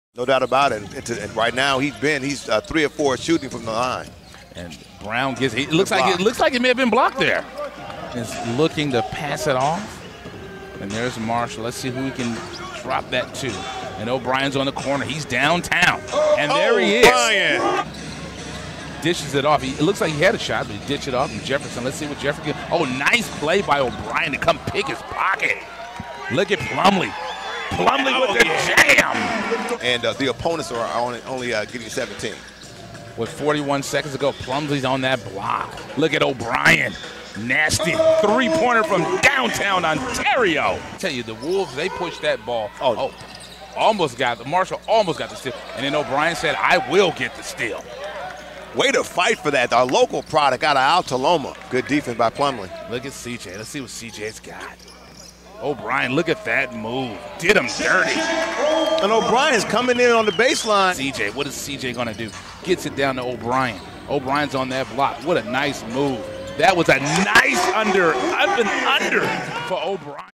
Agua Caliente Clippers Highlights | JJ O'Brien (10 points) Game Highlights vs. Iowa Wolves
JJ O'Brien (10 points) Game Highlights vs. Iowa Wolves